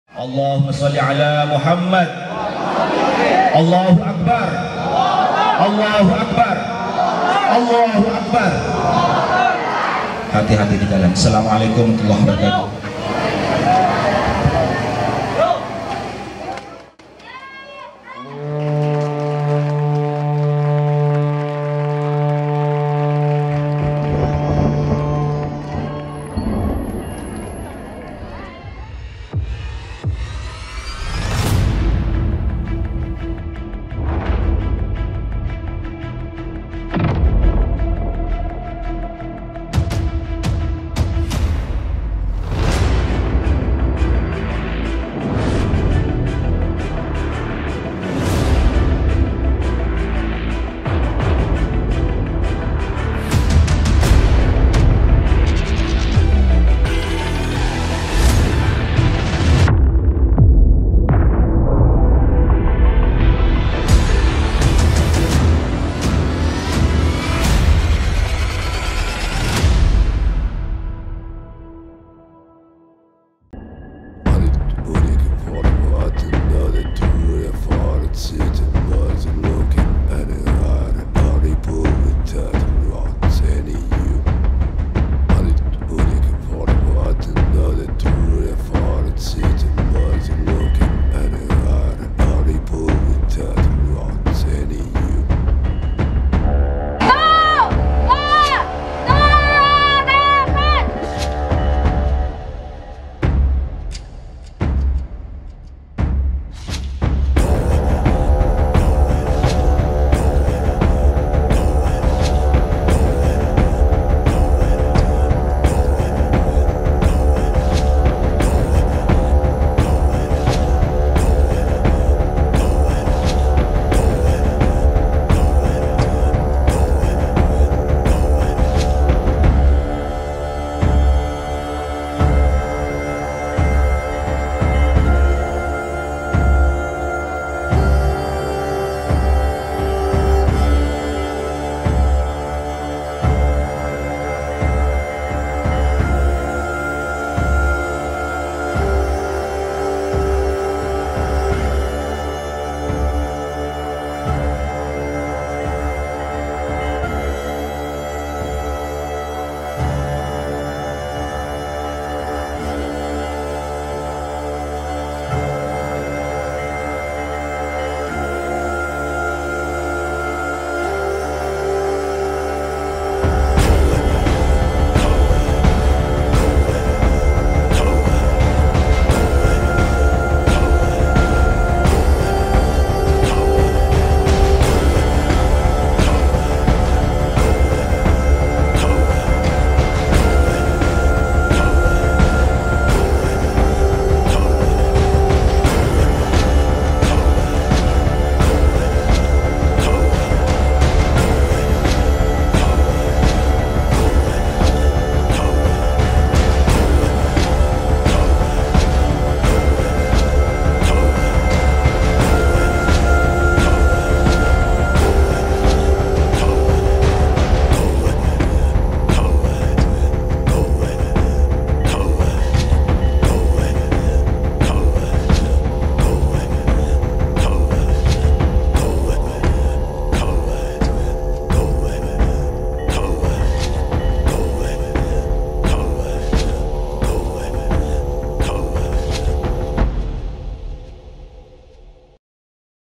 KIRAB KARNAVAL SANTRI PONDOK PESANTREN NURUL AMIN
KIRAB KARNAVAL SANTRI PONDOK PESANTREN NURUL AMIN  DALAM RANGKA HARLAH KE-35